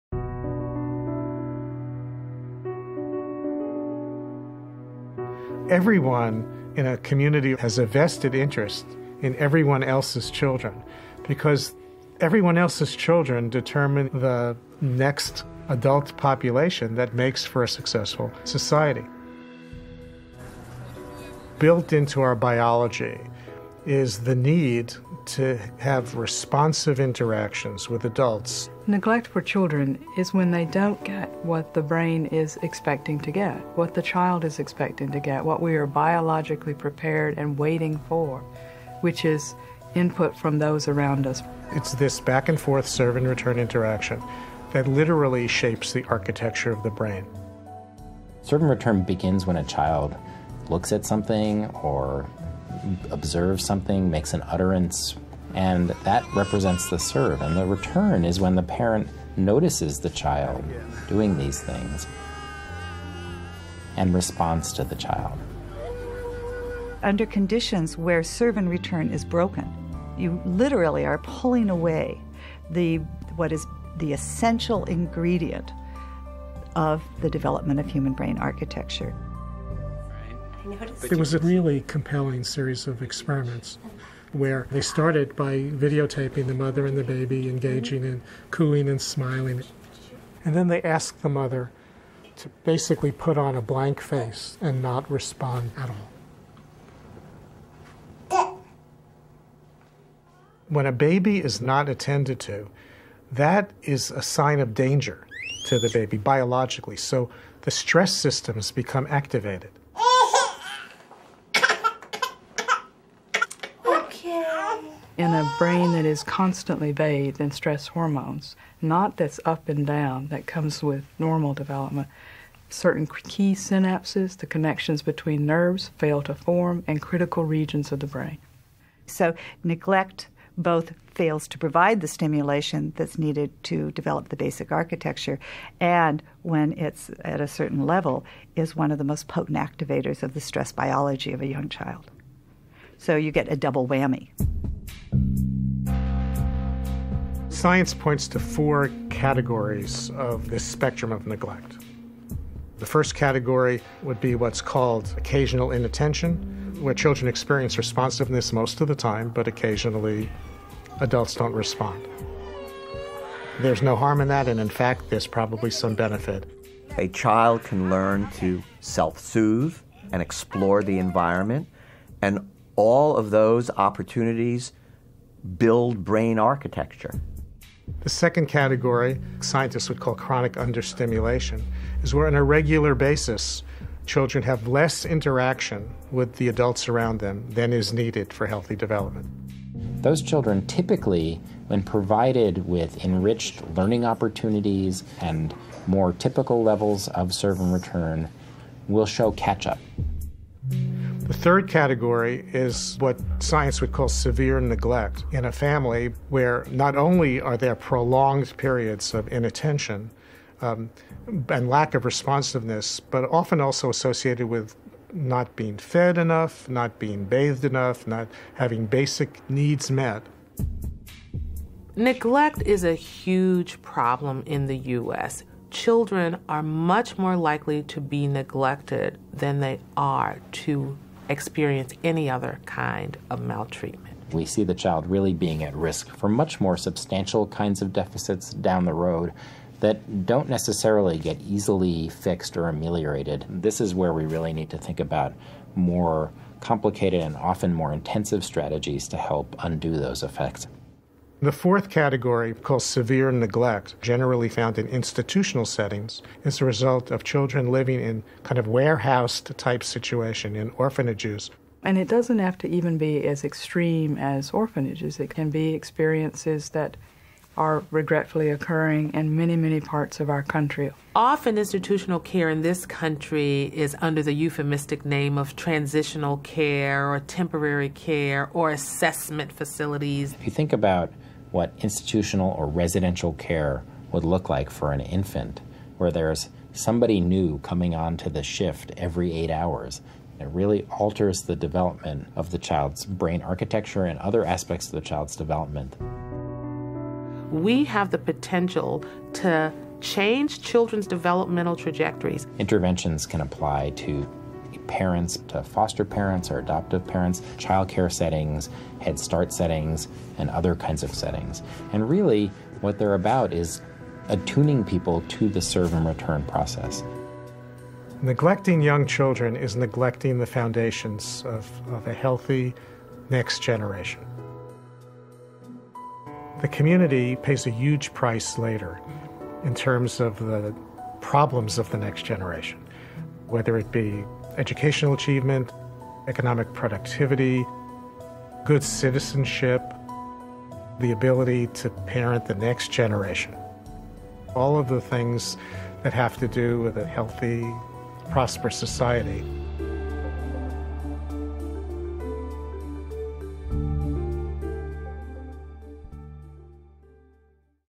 InBrief: The Science of Neglect (Serbian)
Learn more about the science of neglect on our website (page in English)

This video, subtitled in Serbian, explains the extensive biological and developmental research that shows significant neglect—the ongoing disruption or significant absence of caregiver responsiveness—can cause more lasting harm to a young child's development than overt physical abuse. These effects can include subsequent cognitive delays, impairments in executive functioning, and disruptions of the body's stress response. This edition of the InBrief series explains why significant deprivation is so harmful in the earliest years of life and why effective interventions are likely to pay significant dividends in better long-term outcomes in learning, health, and parenting of the next generation.

This 6-minute video provides an overview of The Science of Neglect: The Persistent Absence of Responsive Care Disrupts the Developing Brain, a Working Paper from the National Scientific Council on the Developing Child.